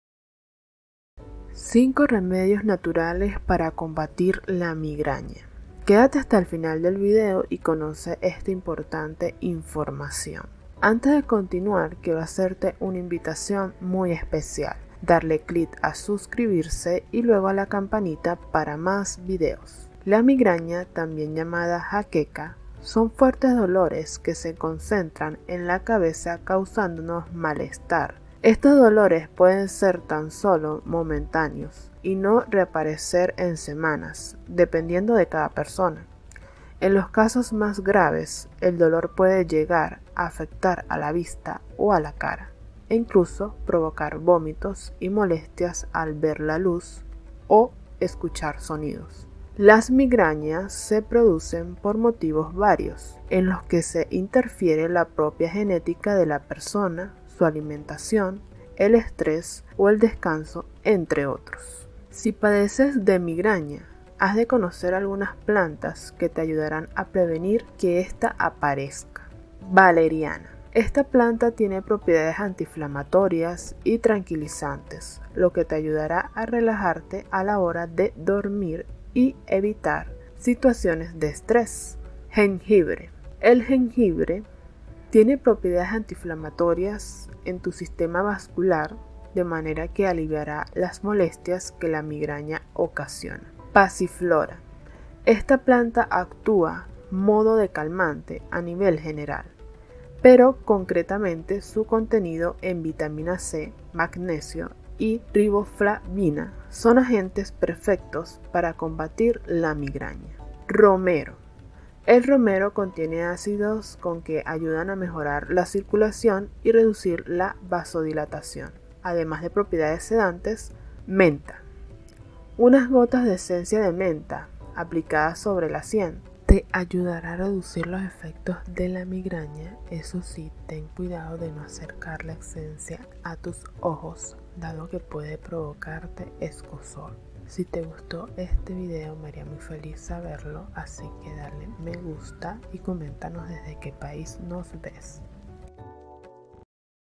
5 Remedios naturales para combatir la migraña
Una migraña, también conocida como migraña, es un dolor intenso centrado en la cabeza que nos hace sentir incómodos. Estos dolores pueden ser solo temporales y pueden no repetirse durante varias semanas, según el individuo. En los casos más severos, el dolor puede afectar la visión o la cara, e incluso puede causar vómitos y molestias al ver luces o escuchar sonidos.
Los dolores de cabeza por migraña ocurren por una variedad de razones, entre las que pueden interferir factores como la propia genética de una persona, su dieta, el estrés o el descanso.
Si sufres de migrañas, debes conocer algunas plantas que pueden ayudarte a prevenirlas.
NOTA: VIDEO INFORMATIVO, NO SUSTITUYE LA OPINION DE UN MEDICO